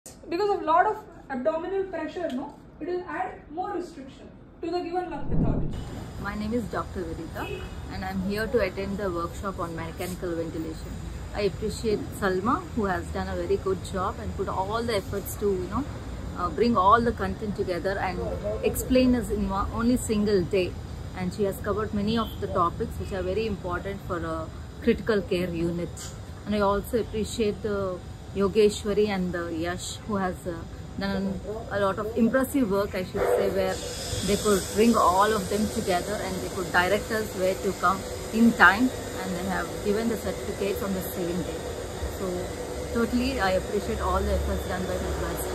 Mechanical Ventilation Training | Medvarsity
Simulation-based training allows healthcare professionals to refine their skills in managing patients requiring mechanical ventilation.

The recent Mechanical Ventilation workshop held at Medvarsity on 27th Dec 2023, offered a hands-on and immersive approach to understanding the complexities of respiratory support.

We thank Dr Salma for providing invaluable insights into mechanical ventilation that allow healthcare professionals to hone their decision-making skills and enhance their ability to respond effectively to complex clinical situations.